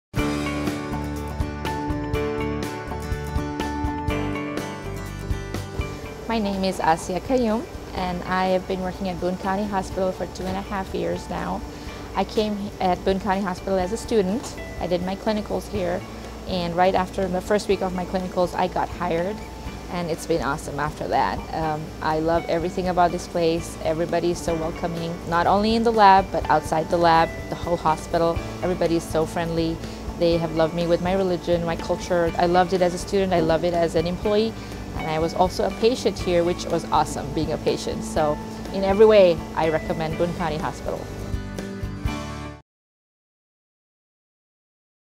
ASIYA TESTIMONIAL
Employee testimonial about working at Boone County Hospital in Boone, Iowa